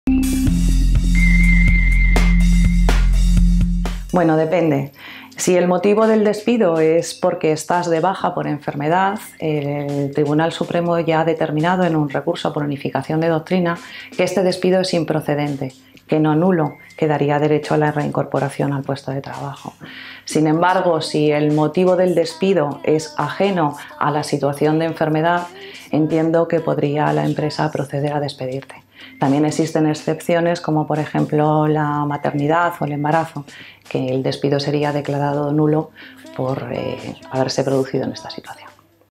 Consultas Laborales: ¿Te pueden despedir estando de baja?
¿Te pueden despedir estando de baja? Pilar responde a una de vuestras consultas